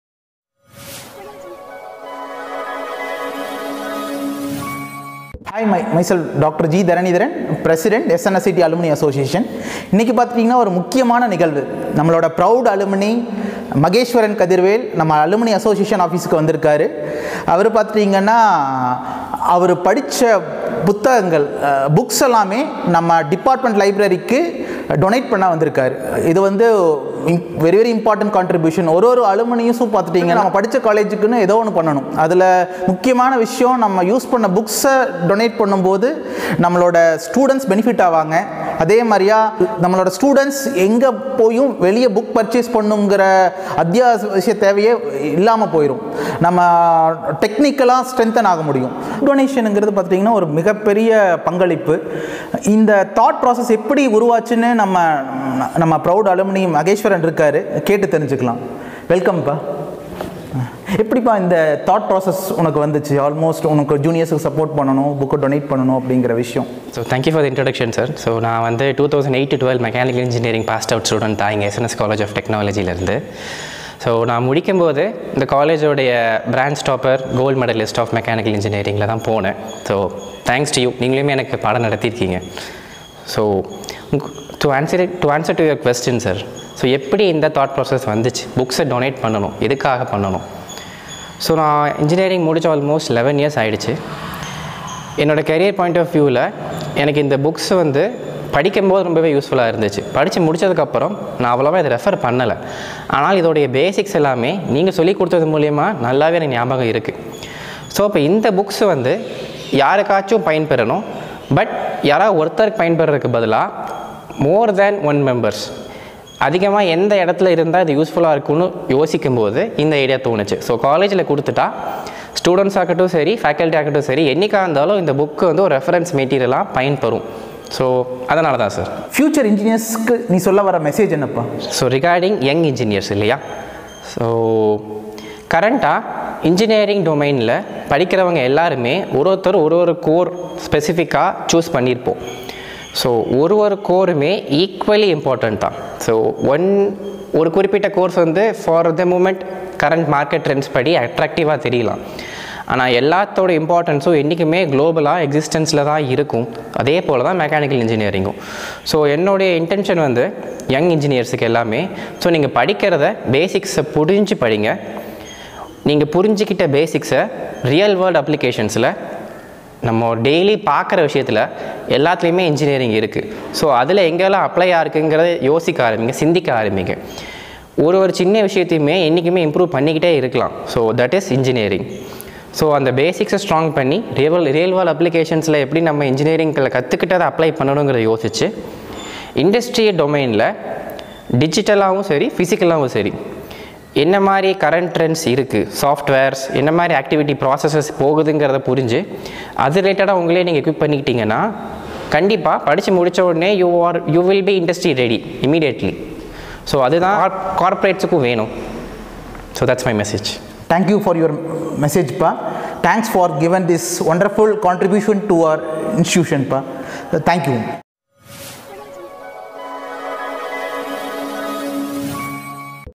Books Donated by Alumni Maheswaran Kathirvel Mech | SNS College of Technology | SNS Institutions
Our Proud Alumni Maheswaran Kathirvel Mech (2008–12 batch) The project leader of Sacha Engineering donated valuable textbooks to the SNSCT Library through the SNSCT Alumni Association.